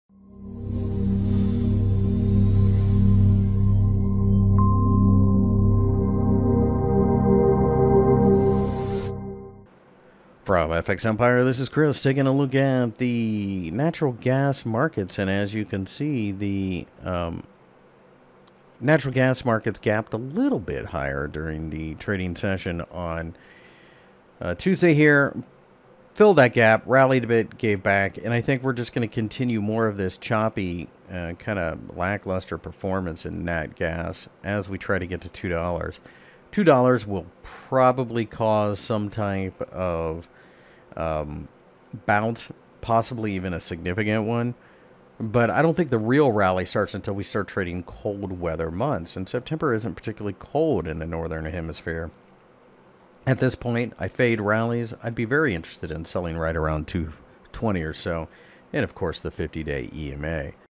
Natural Gas Technical Analysis for August 07, 2019 by FXEmpire
Natural gas markets gapped a bit higher to the upside on Tuesday, but then pulled back to fill that gap before rallying... For the full article go - Natural Gas Forecast August 07, 2019, Technical Analysis.